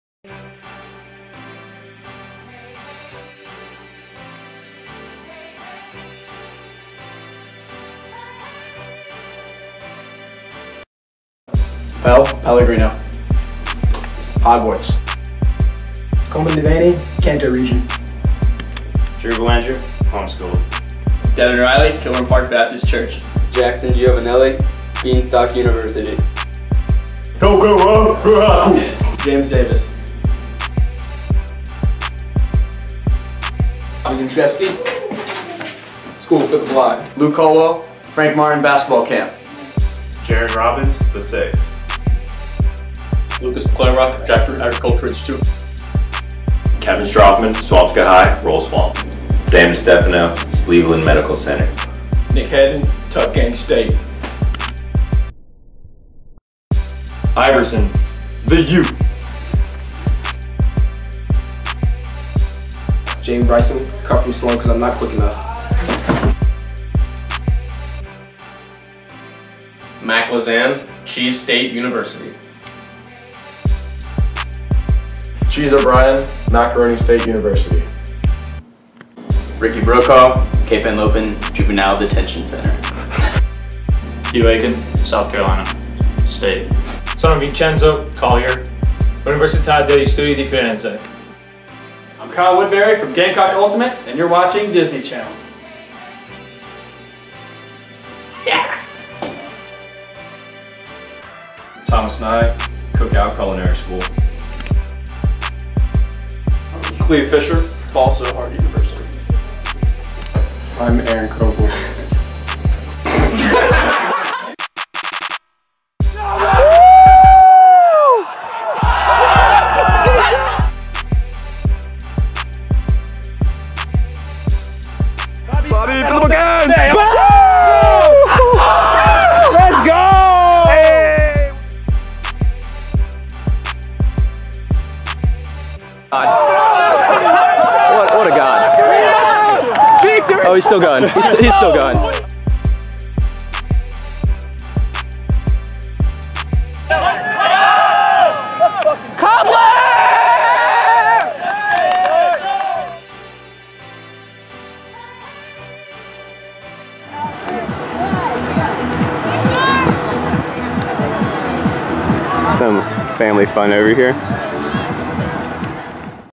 Gamecock Ultimate: 2020 Intro
Meet the 2020 edition of South Carolina Gamecock Ultimate. Let us tell you who we are and where we're from, with a little help from a green towel, a kitchen, and some poor screen caps.